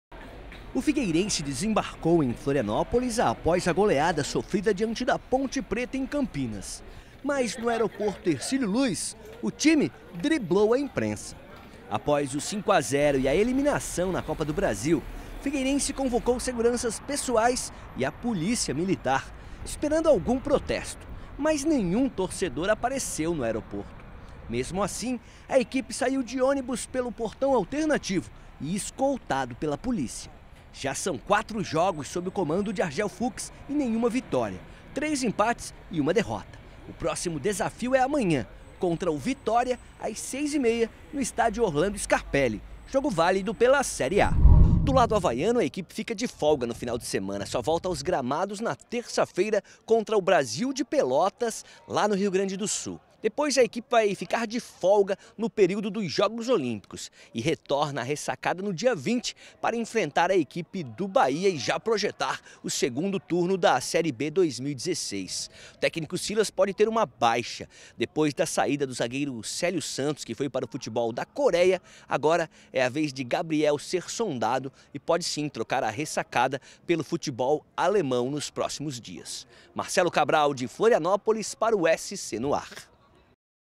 Após derrota Figueirense não atende imprensa e Avaí fica de folga no fim de semana
Após a derrota e a eliminação da Copa do Brasil, o time do Figueirense retorna a capital e não atende a imprensa. Do lado avaiano a equipe fica de folga no fim de semana e joga somente na terça-feira contra o Brasil de Pelotas, no Rio Grande do Sul.